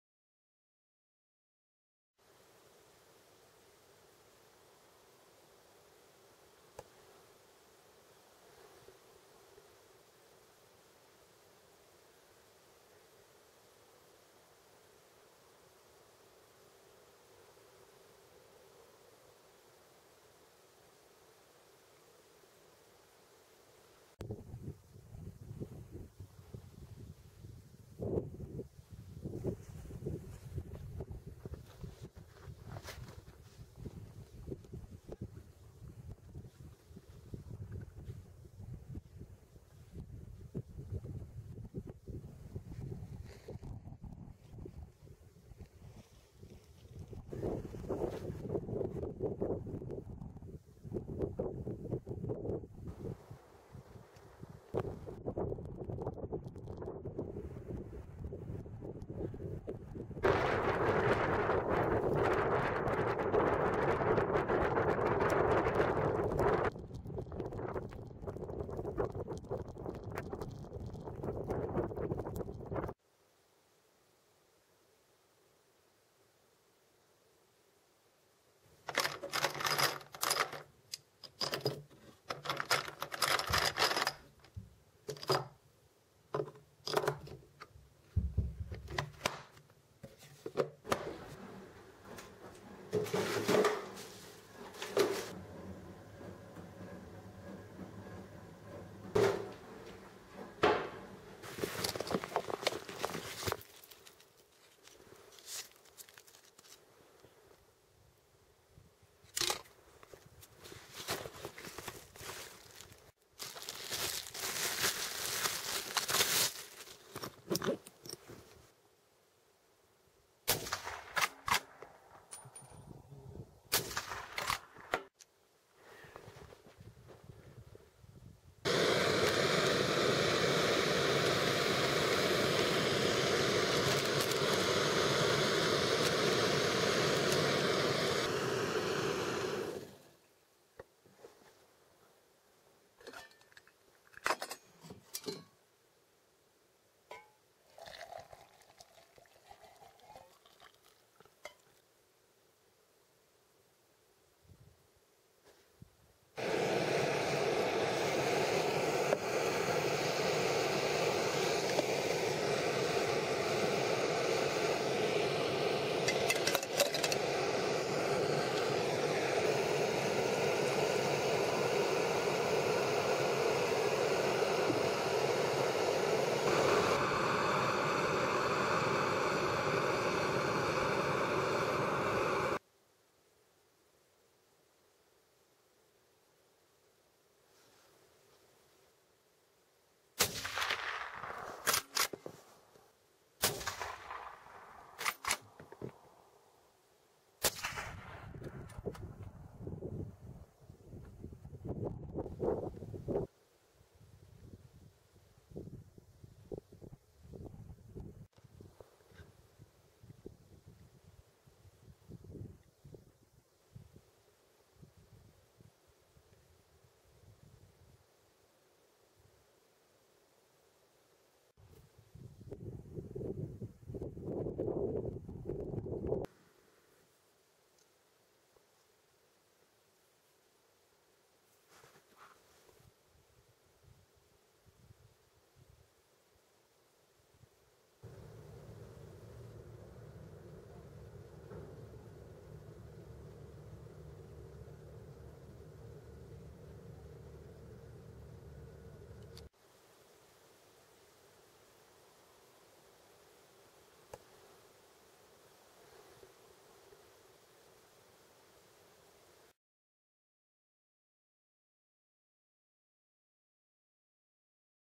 Longrange blog 40: Frozen ammo
This was a failed experiment as I couldn't measure temperature good enough. To shoot successfully longrange in all conditions you need to know your V0. Normally I just log this throughout the year, but this time I decided on a little experiment to see if I could get an indication of what 1 year of velocity data would be... Conclusion: Simulating warmer weather was a lot easier than colder...